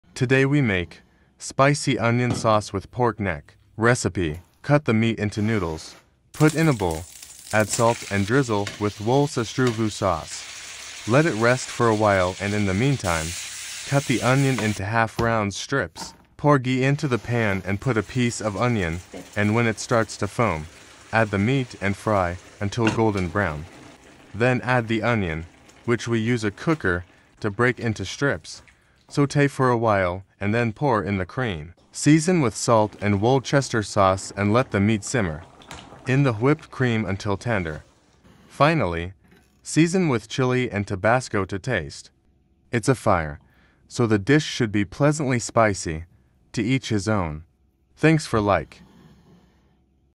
🌶️SPICY ONION SAUCE WITH PORK NECK🌶️
⚡️RECIPE⚡️
👉 Cut the meat into noodles, put in a bowl, add salt and drizzle with wolčestrovou sauce. Let it rest for a while and in the meantime, cut the onion into half rounds (strips).

👉Pour ghee into the pan and put a piece of onion and when it starts to foam, add the meat and fry until golden brown. Then add the onion, which we use a cooker to break into strips, saute for a while and then pour in the cream.

Season with salt and wolcester sauce and let the meat simmer in the whipped cream until tender. Finally, season with chilli and Tabasco to taste. It's a fire, so the dish should be pleasantly spicy. To each his own.

⚡️5 PORTIONS⚡️
👉PREP WORK: 10 MINUTES
👉TOTAL PREPARATION TIME: 35 MINUTES

⚡️INGREDIENTS:⚡️
👉1000g Pork neck 🥩
👉300g Onion 🧅
👉400g cream 35%🥛
👉2tbsp wolčestru
👉2tsp salt🧂
👉1tsp pepper
👉Chilli to taste🌶️

⚡️NUTRITION PER SERVING⚡️
(1 PORTIONS): total carbohydrates: 9.3g | net carbohydrates: 8g | protein: 42g l fat: 60,9g | calorific value: 751 kcal